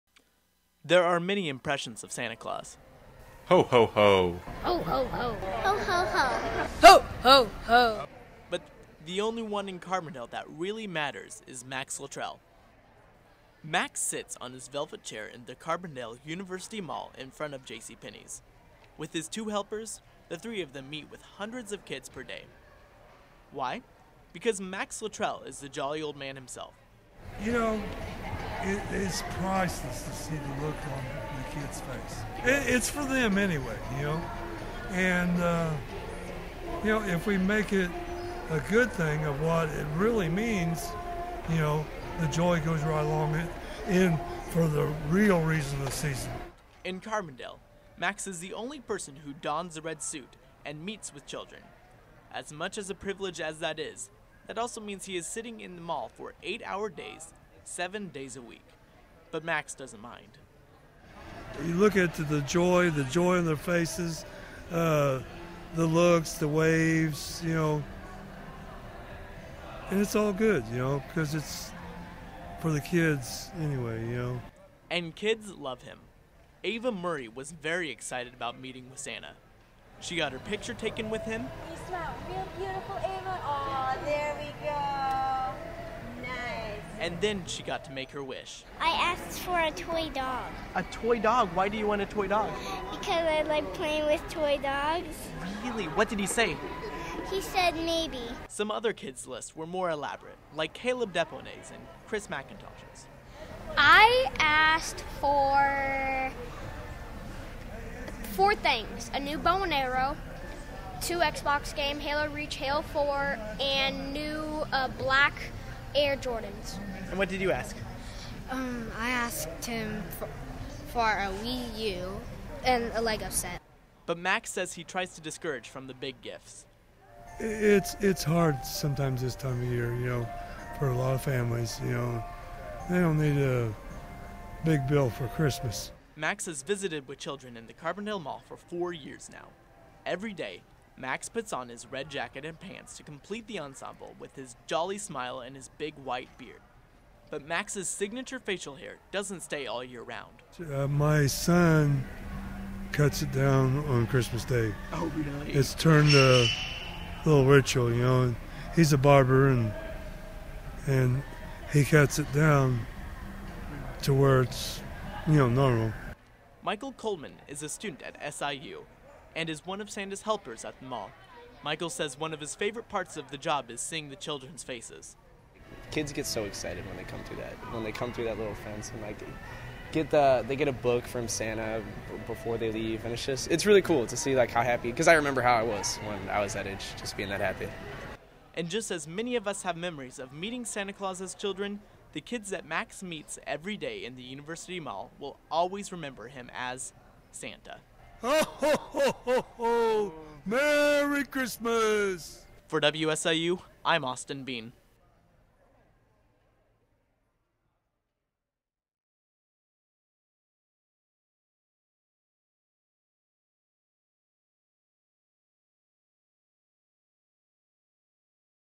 The Jolly Old Man in Carbondale - A Radio Project
Tis' the season for Christmas trees, presents, joy and cheer... and one jolly old man!

Santa Clause is one of the biggest symbols of this holiday season. And he can be sighted all around the world this month.

I spoke to Carbondale's own Santa to really get to know the man behind the beard for my feature radio story for the Audio Journalism for Electronic Media class at Southern Illinois University.

Just a note... those with young ears may want to step away!